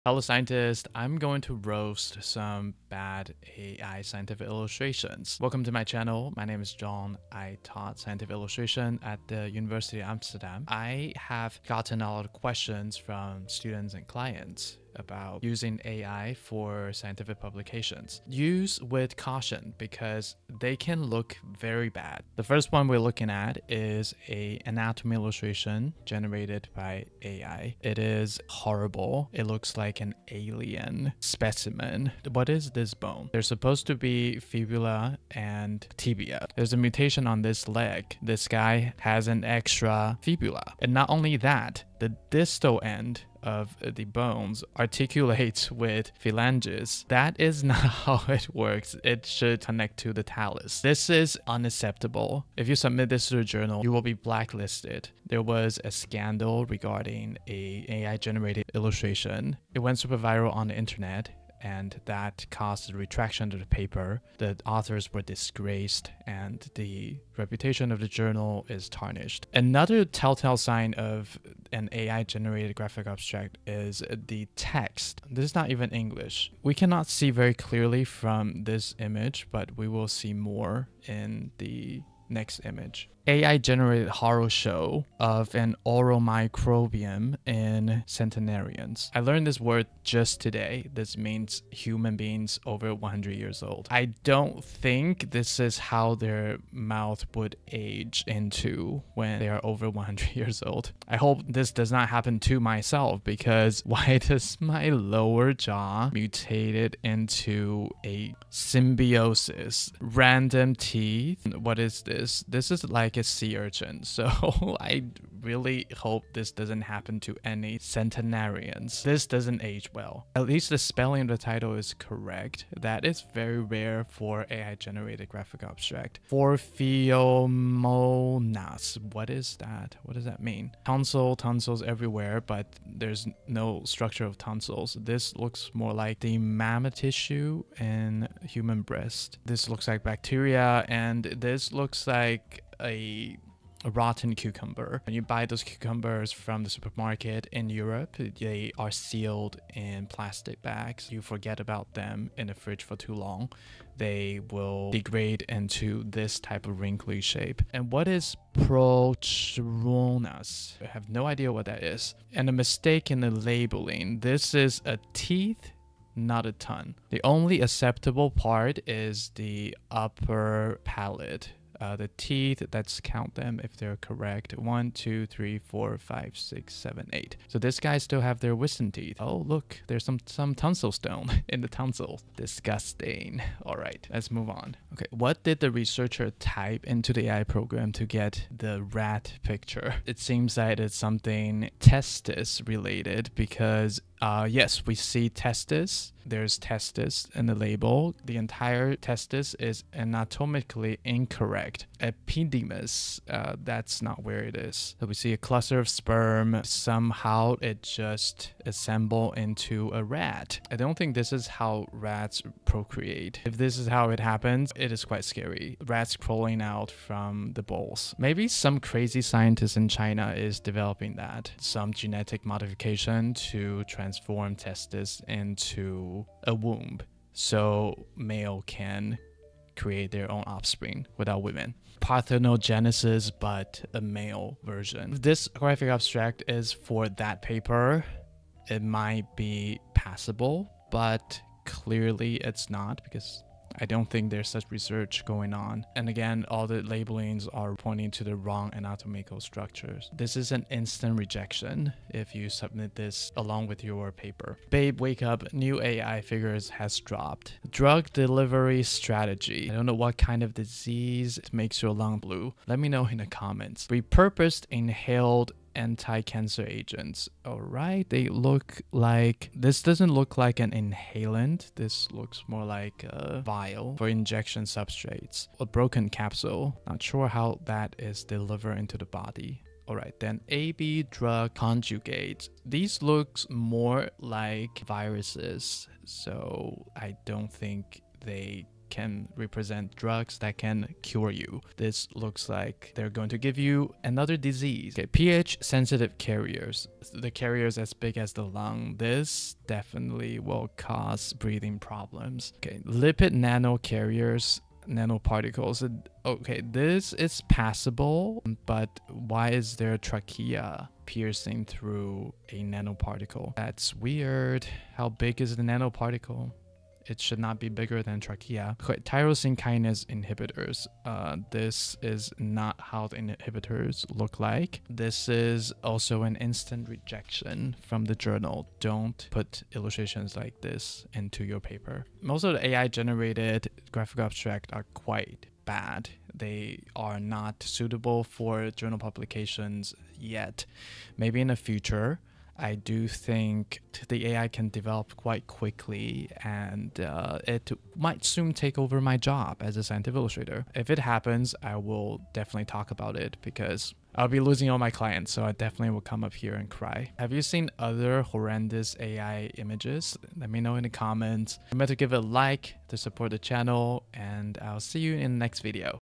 Bad AI Art is Taking Over Science
Roasting bad AI graphical abstracts

shorturl.at/cdK05